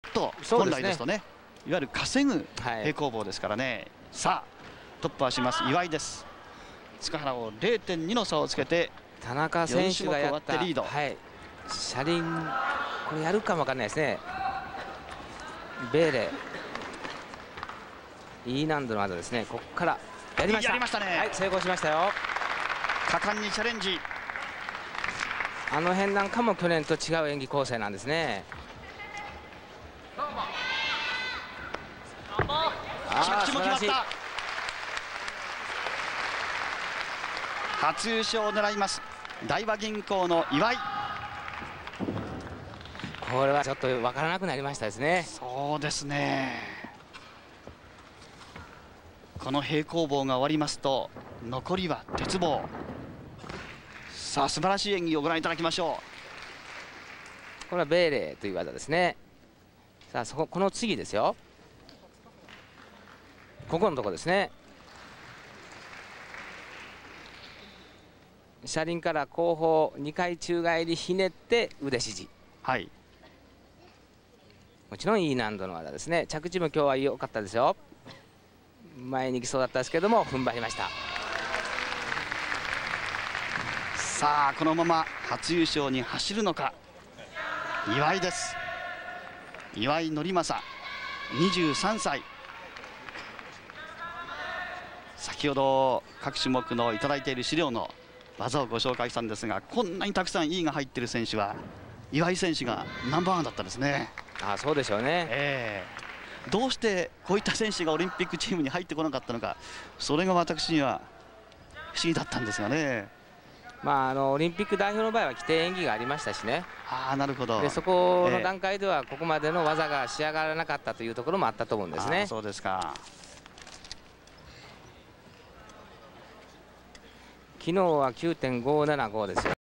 Norimasa Iwai PB 1996
The 50th All Japan Artistic Gymnastics Championship